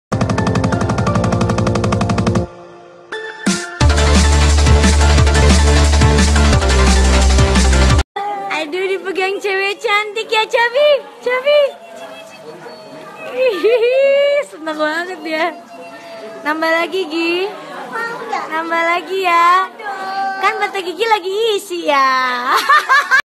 Yeee, Baby Arsha Lagi Digendong Tante Nagita yang Cantik
Wah, Tante Nagita lagi main sama Arsya nih, Arsyanya juga gak rewel di gendong Tante Nagita.